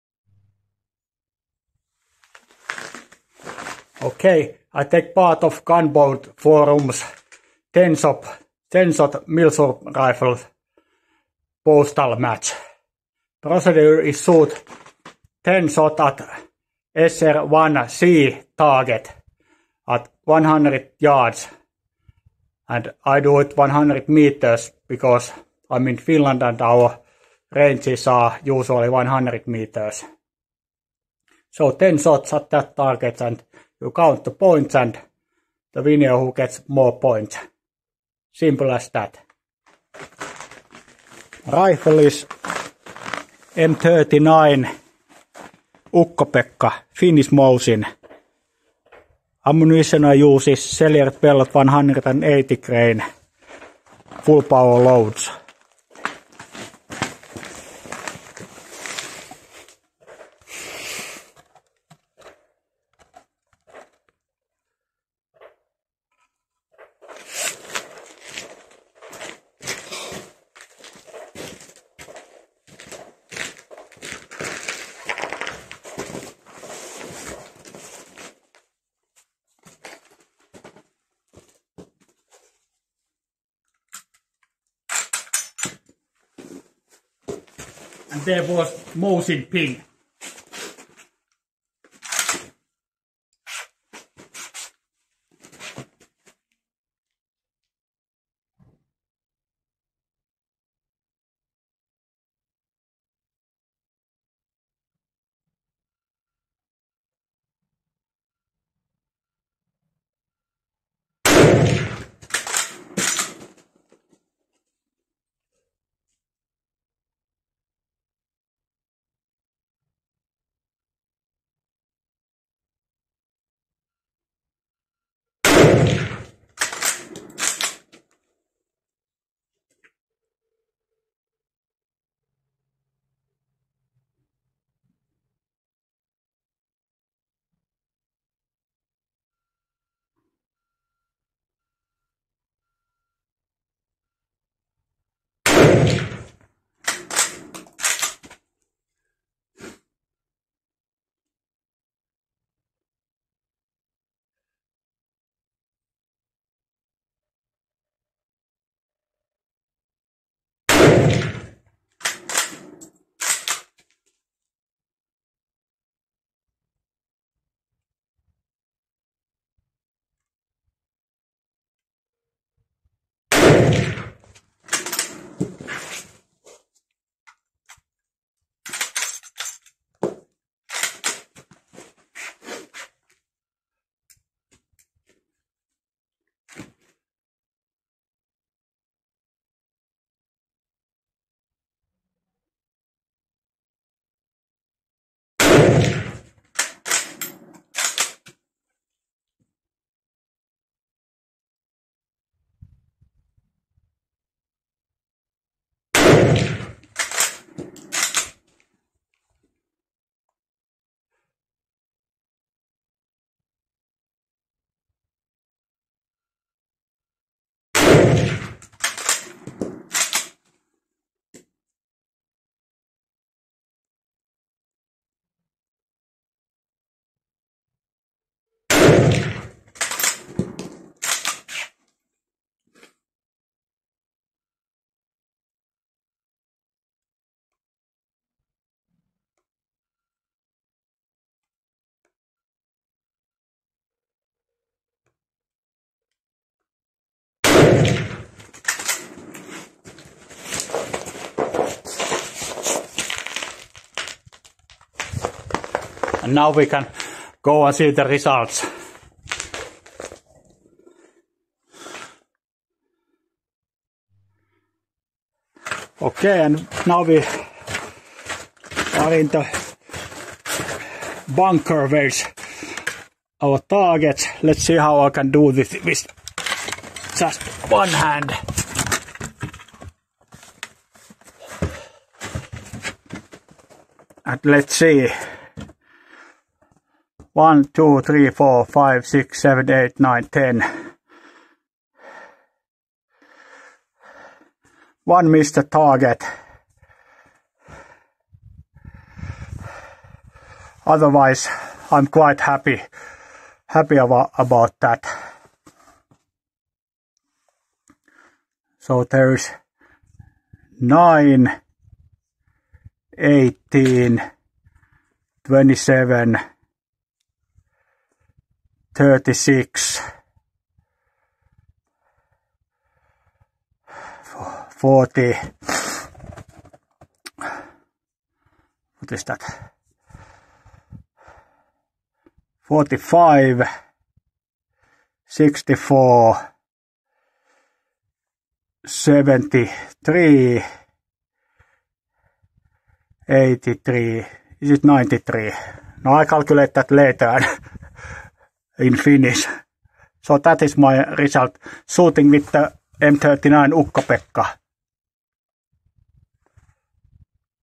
M/39 Ukko-Pekka Gunboards forums postal match.
My take on Gunboards forums winter postal match. WW2 era or earlier iron sight rifle. 10 shots at 100yd or 100 meters. Target is sr-1c. Shooting is unsupported prone or unsupported bench. I used 7,62x53r Finnish Mosin m/39 Ukko-Pekka rifle and Sellier&Bellot 180gr FMJ ammo (7,62x54r).